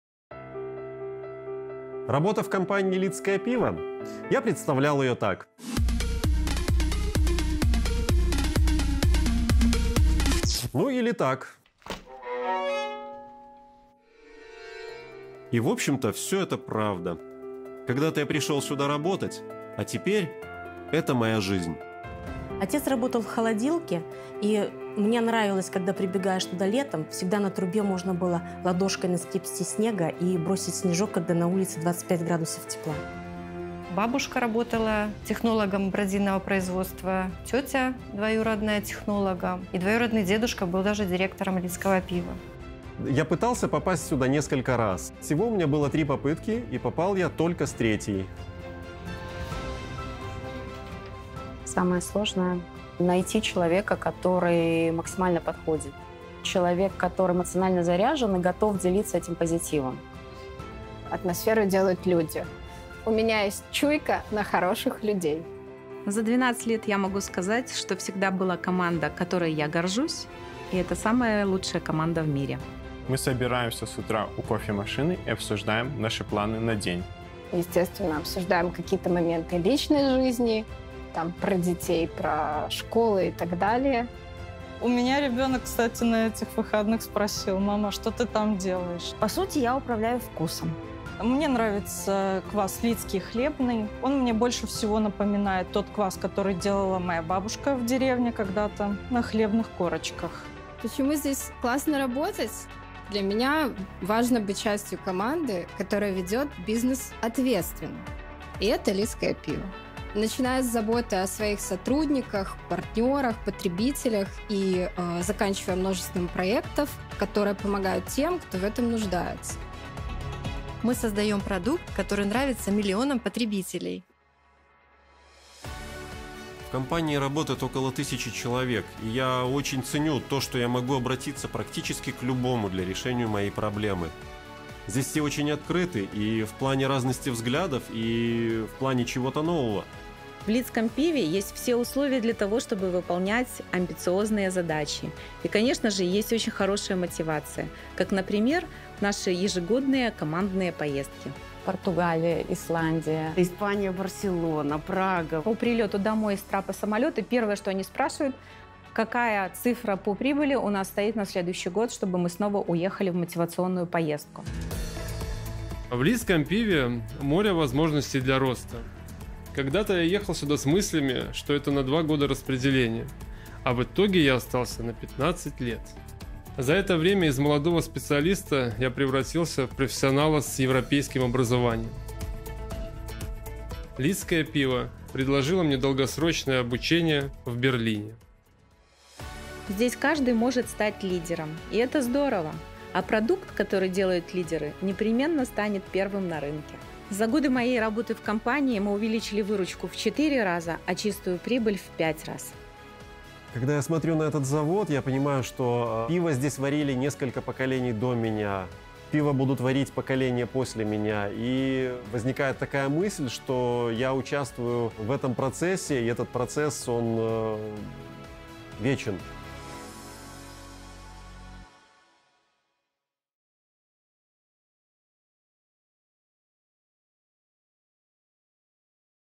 "Лидское: добро пожаловать в семью" HR-ролик
Ролик о работе на одном из крупнейших предприятий пивоваренной отрасли Беларуси "Лидское пиво". Люди разных профессий честно рассказывают, как представляли себе этот завод и каким он оказался. И, разумеется, о любви к пенному напитку:)